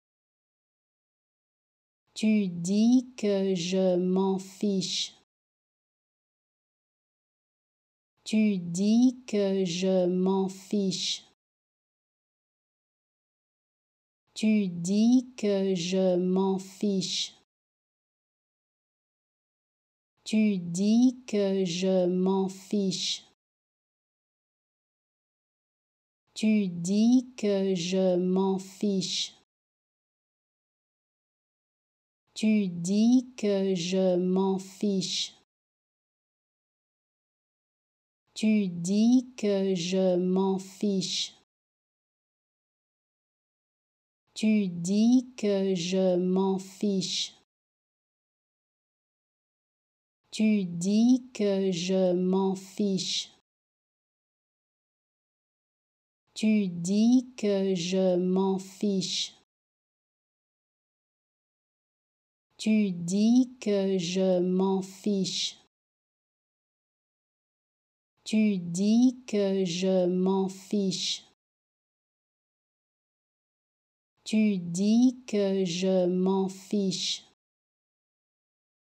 How to Pronounce ''Tu dis que je m'en fiche'' (You say I don't care) Correctly in French
Learn how to say and properly pronounce ''Tu dis que je m'en fiche'' in French with this free pronunciation tutorial. Apprenez à prononcer des mots en français grâce à nos tutoriels.